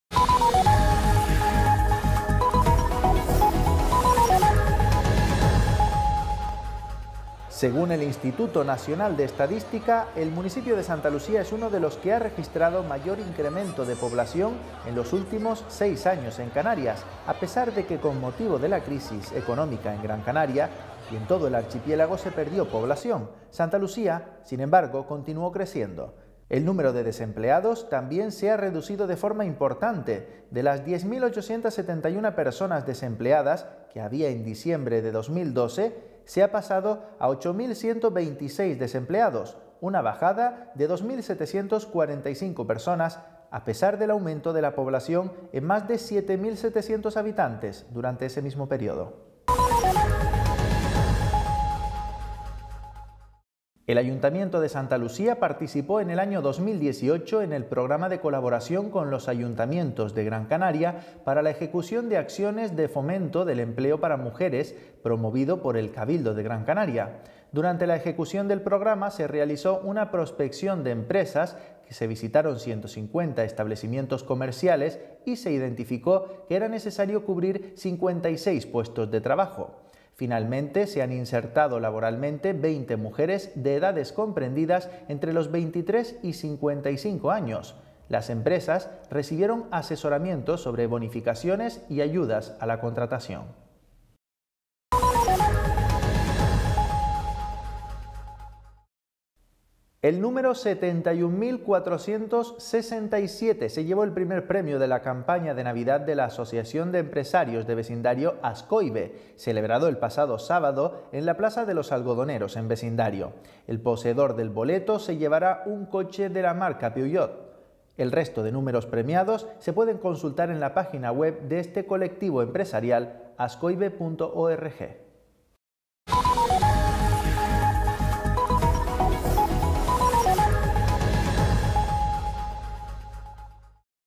INFORMATIVO SANTA LUCÍA WEB - 16 01 2019
Informativo del municipio de Santa Lucía:
1-Aumento de la población y descenso del paro en Santa Lucía.
2-Asesoramiento para la bonificación de empresas en Santa Lucía.
3-Sorteo de la campaña de Navidad de ASCOIVE.